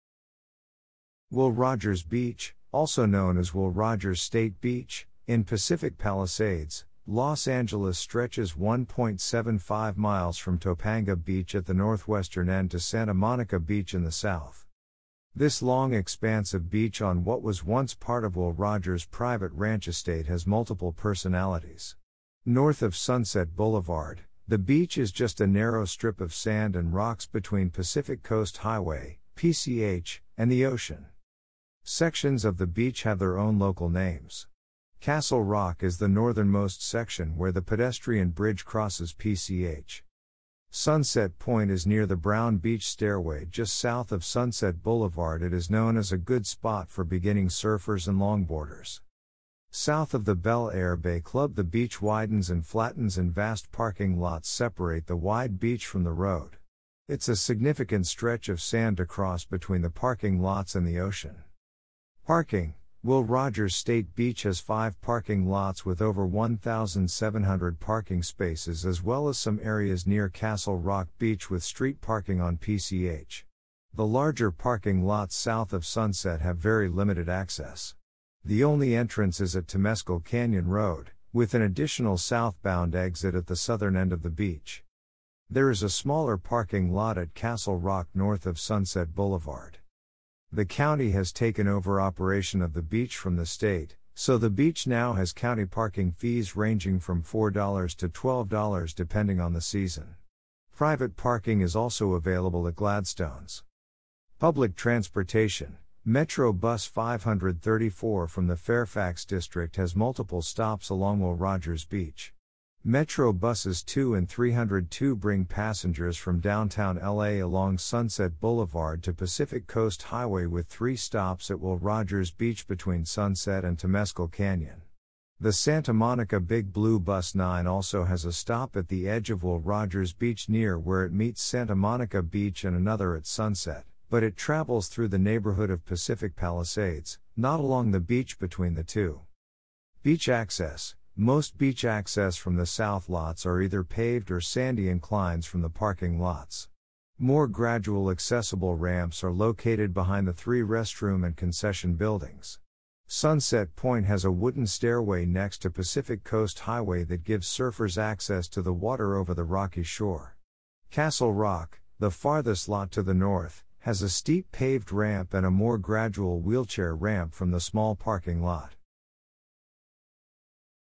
Will Rogers Beach
Will Rogers Beach, also known as Will Rogers State Beach, in Pacific Palisades, Los Angeles stretches 1.75 miles from Topanga Beach at the northwestern end to Santa Monica Beach in the south..
This long expanse of beach on what was once part of Will Rogers' private ranch estate has multiple personalities. North of Sunset Blvd, the beach is just a narrow strip of sand and rocks between Pacific Coast Highway (PCH) and the ocean..
Sections of the beach have their own local names. Castle Rock is the northernmost section where the pedestrian bridge crosses PCH..
Sunset Point is near the brown beach stairway just south of Sunset Blvd. It is known as a good spot for beginning surfers and longboarders..
South of the Bel Air Bay Club the beach widens and flattens and vast parking lots separate the wide beach from the road. It's a significant stretch of sand to cross between the parking lots and the ocean..
Parking: Will Rogers State Beach has five parking lots with over 1700 parking spaces as well as some areas near Castle Rock Beach with street parking on PCH. The larger parking lots south of Sunset have very limited access. The only entrance is at Temescal Canyon Road, with an additional southbound exit at the southern end of the beach..
There is a smaller parking lot at Castle Rock north of Sunset Blvd..
The county has taken over operation of the beach from the state, so the beach now has county parking fees ranging from $4 to $12 depending on the season. Private parking is also available at Gladstone's..
Public Transportation: Metro Bus 534 from the Fairfax District has multiple stops along Will Rogers Beach. Metro Buses 2 and 302 bring passengers from Downtown LA along Sunset Blvd to Pacific Coast Highway with three stops at Will Rogers Beach between sunset and Temescal Canyon. The Santa Monica Big Blue Bus 9 also has a stop at the edge of Will Rogers Beach near where it meets Santa Monica Beach and another at Sunset, but it travels through the neighborhood of Pacific Palisades, not along the beach between the two..
Beach Access: Most beach access from the south lots are either paved or sandy inclines from the parking lots. More gradual accessible ramps are located behind the three restroom and concession buildings. Sunset Point has a wooden stairway next to Pacific Coast Highway that gives surfers access to the water over the rocky shore. Castle Rock, the farthest lot to the north, has a steep paved ramp and a more gradual wheelchair ramp from the small parking lot..